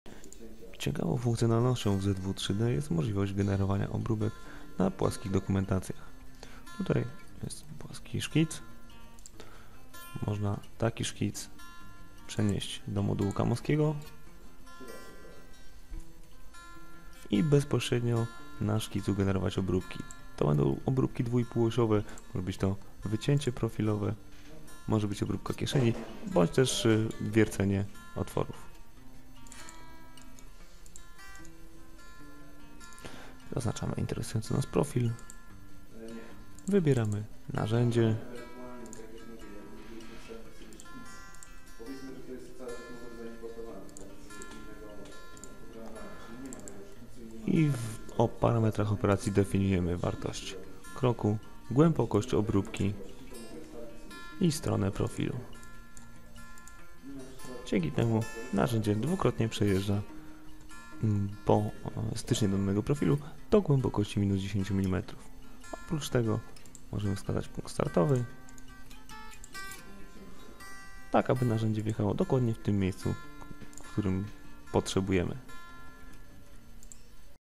Obróbka profilu w 2 osiach
Generowanie frezowania na płaskich plikach za pomocą kilku kliknięć. Już moduł 2x Machining programu ZW3D pozwala na tworzenie takich operacji jak wycięcie po profilu, obróbka kieszeni czy wiercenie.